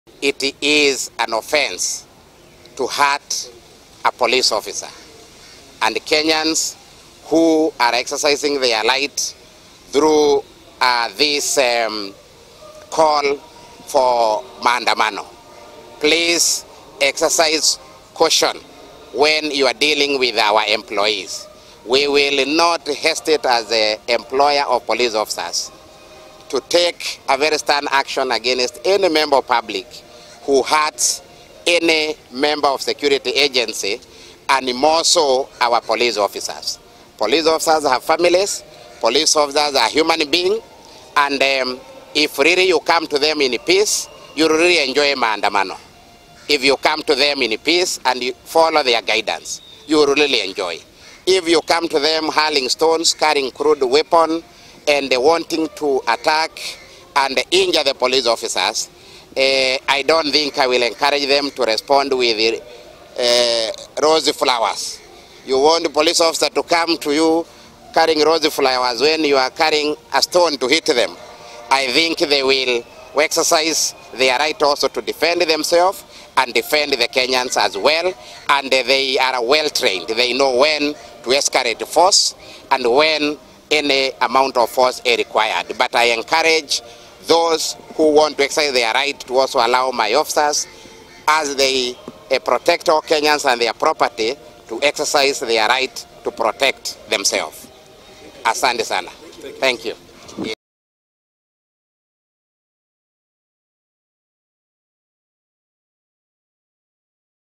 Police commission warns Azimio protestors against attacking police
The National Police Service Commission has warned Azimio supporters against attacking police officers during the Monday demos organized by Raila Odinga.